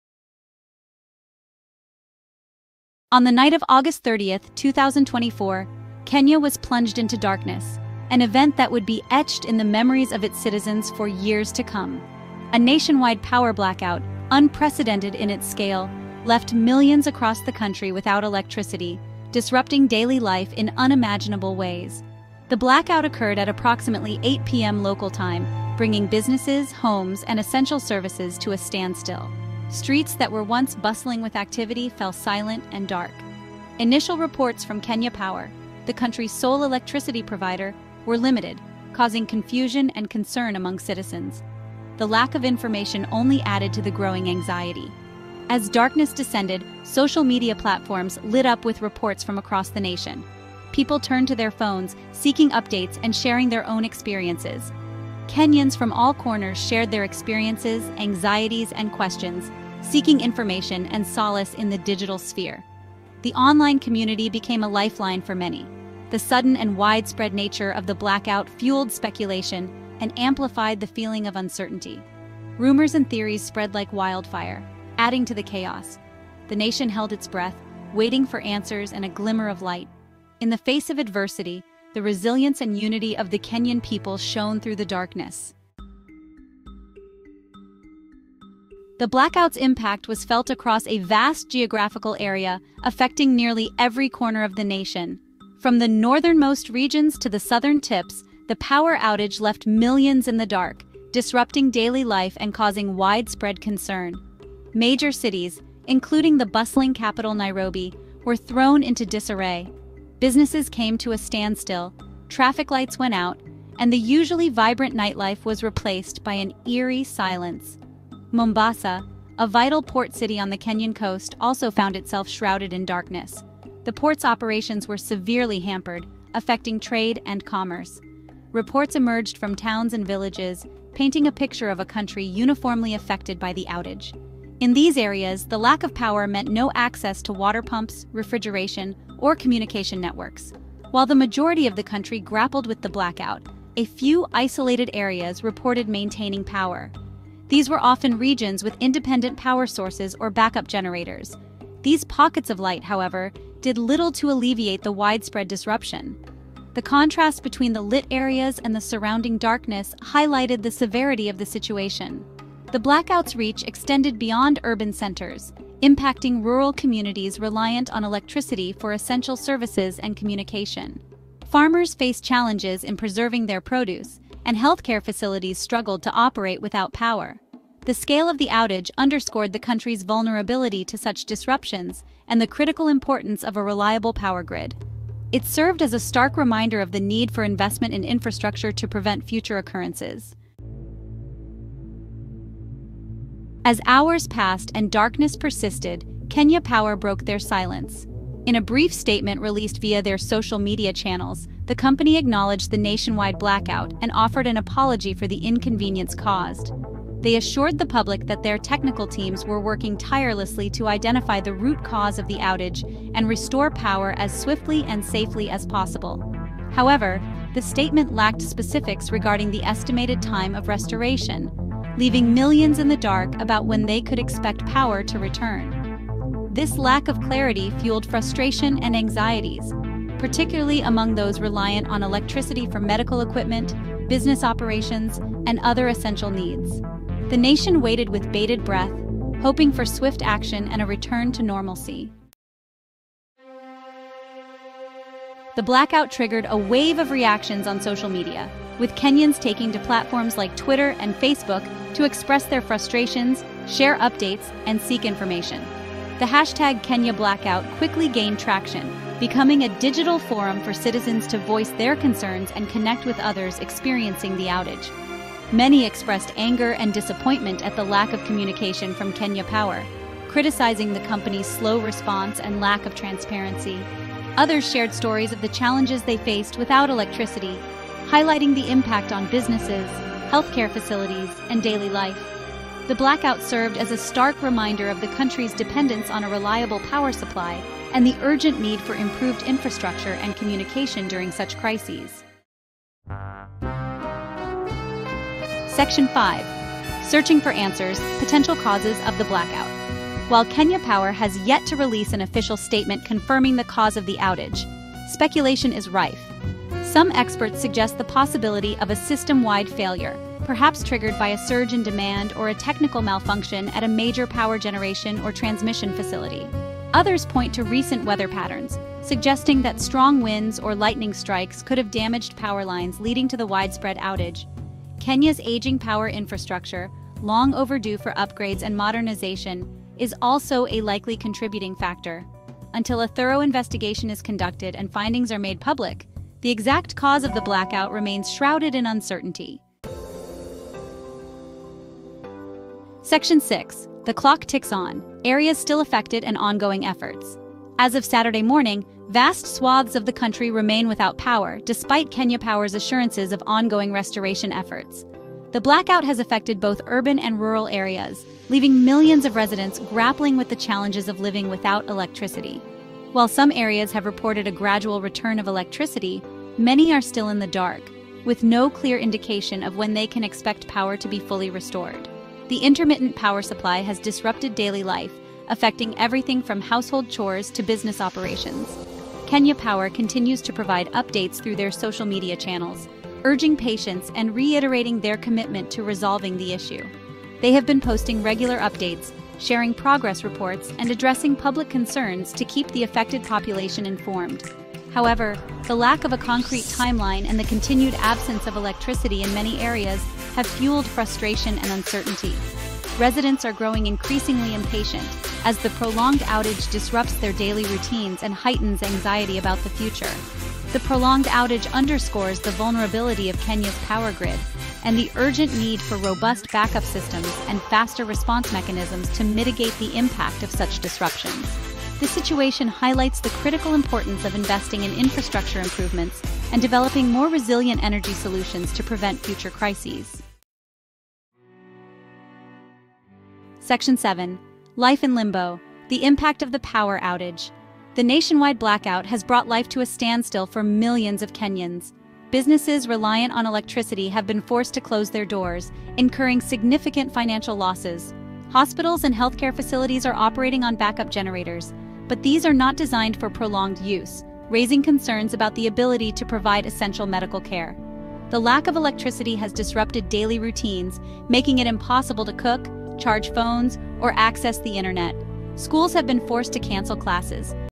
Kenya Power Update: Nationwide Blackout Causes & Customer Reactions! 🇰🇪
Preparing to download
100 %

🚨 **Kenya Power Update: Nationwide Blackout** 🚨

On August 30, 2024, Kenya Power confirmed a widespread electricity outage affecting most regions of the country, leaving many Kenyans frustrated and in the dark! ⚡😕 The North Rift and some parts of the Western region were the only areas not impacted.

In this video, we dive into the latest updates from Kenya Power, their efforts to restore services, and the reactions from citizens across the nation. 📢💬

🔍 **What Happened?**
- **Outage Details:** Most areas remained powerless into Saturday morning.
- **KPLC's Statement:** The company has urged patience but did not disclose the cause of the outage.

💔 **Customer Reactions:**
- Many netizens took to social media to express their frustration, particularly from areas like Westlands and Kirinyaga.

🛠️ **Stay Tuned:**
As we await further updates from KPLC, we'll keep you informed on the situation and any developments regarding the restoration of power.